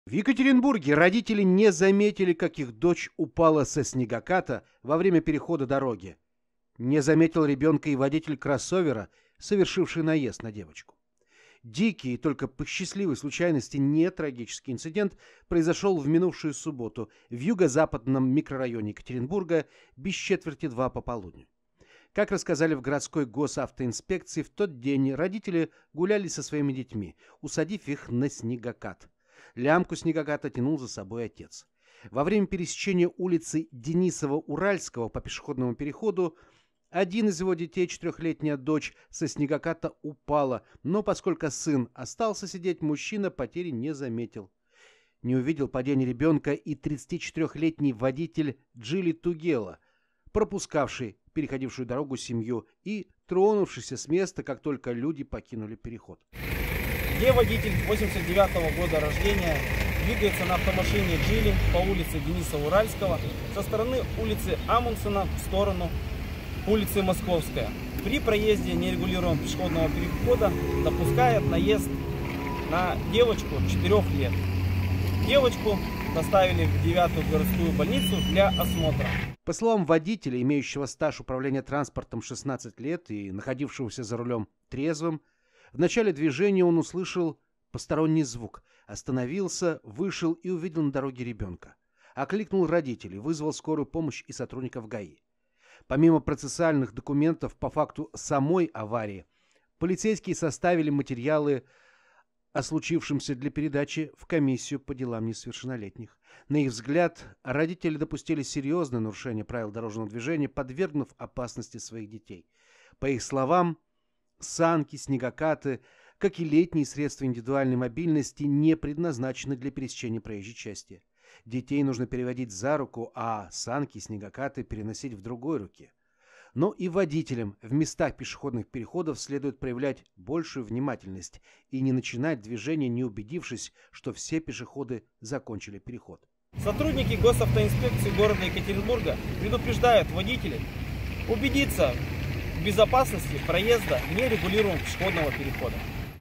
Упавшего на дорогу ребёнка никто не заметил
В Екатеринбурге 4-хлетняя девочка попала под колёса, когда упала со снегоката, на котором её вёз отец через дорогу